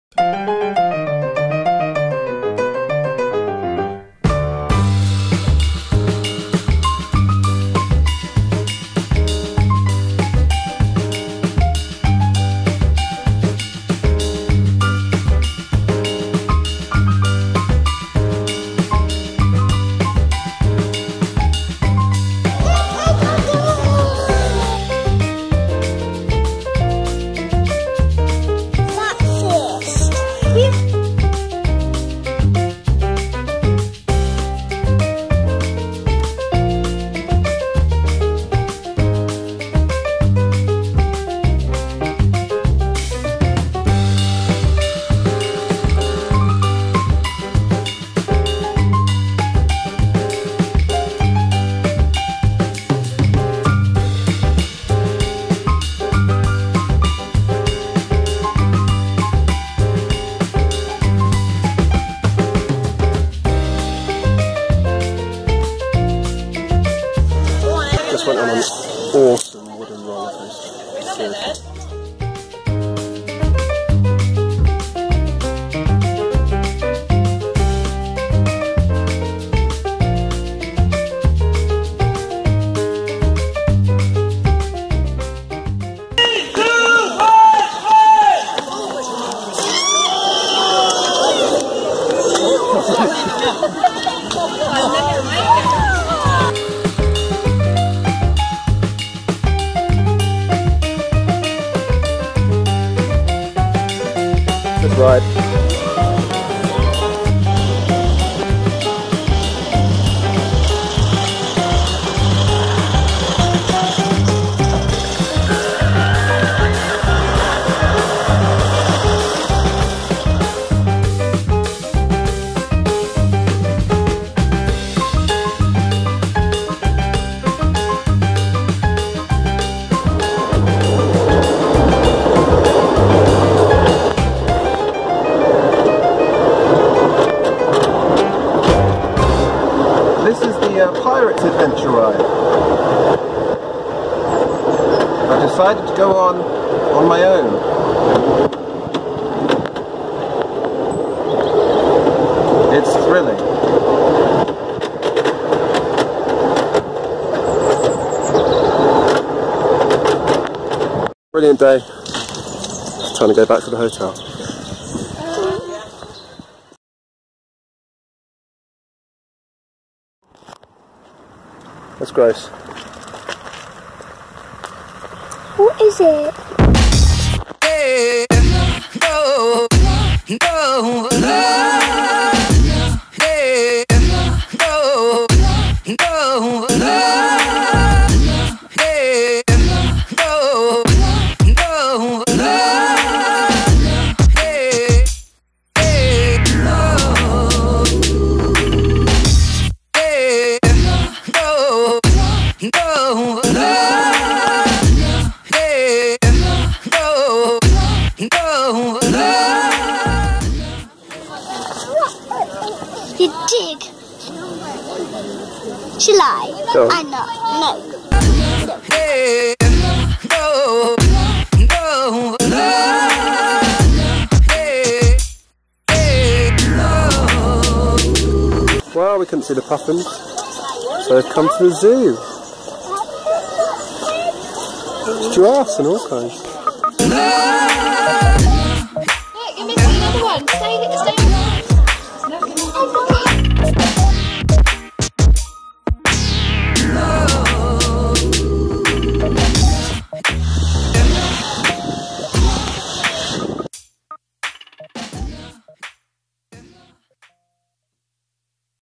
NASTY CATERPILLAR NEST
We spent the weekend in Wales. It was pretty cool. But those caterpillars were nasty. Beautiful, but nasty.